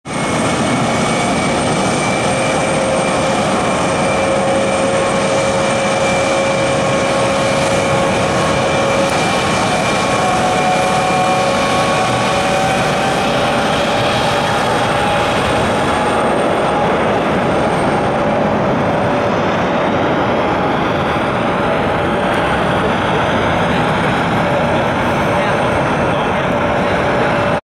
(New livery) Lufthansa 747-8i Take off DUS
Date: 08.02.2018
REG: D-ABYA
Airport: EDDL (Düsseldorf)